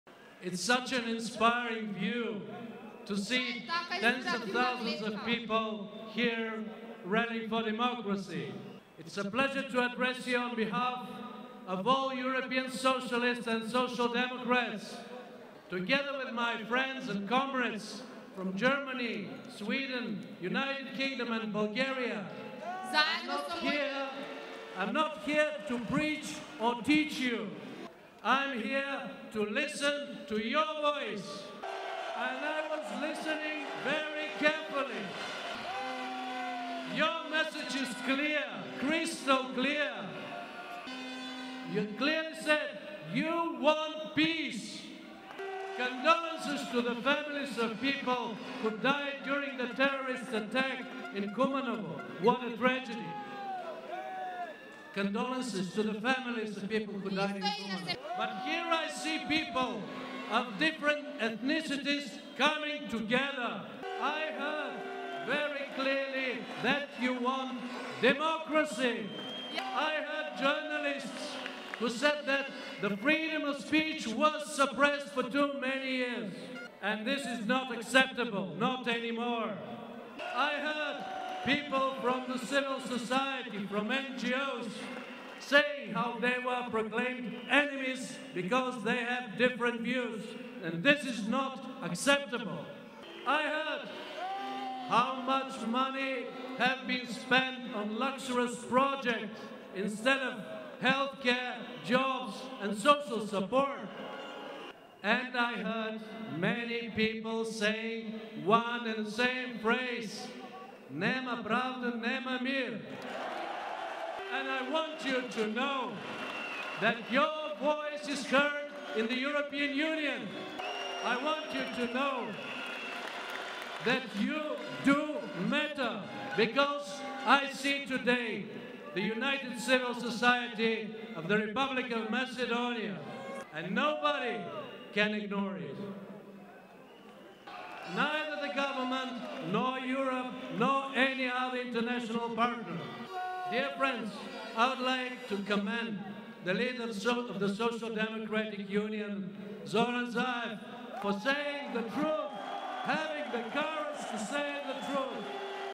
Sergei Stanishev at the Protest in Skopje, 17th of May
Sergei's Stanishev speech at the Protest in Skopje